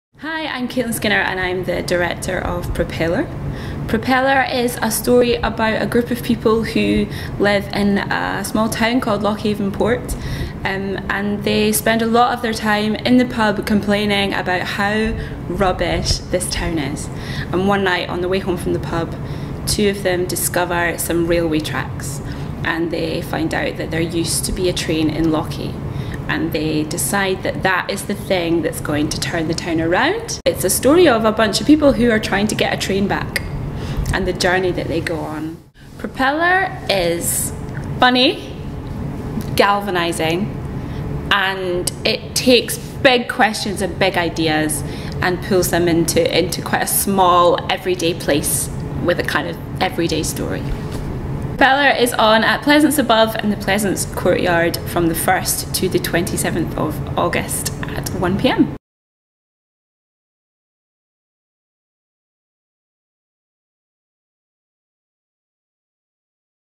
Director Caitlin Skinner chats about the storyline of Propeller
So you want to change the world? Really? You? You Instagram checking, avo-smashing, coconut-flat-white sipping loser? Yeah, the world sucks sometimes – OK, a lot. But you can't do anything about it. Can you? This show says yes, yes you can. Maybe. If you ever feel frustrated about the way things are but don't know what to do about it, this is the show for you. A devised piece about power and the possibility of change, directed by double Fringe First winner Caitlin Skinner and co-created by The Network Ensemble.

See Propeller at The Edinburgh Festival Fringe 2018 at the Pleasance Courtyard (Above), 60 Pleasance, Edinburgh, EH8 9TJ from Wednesday 1st – Monday 27th August 2018 (not 13th or 21st ) at 1pm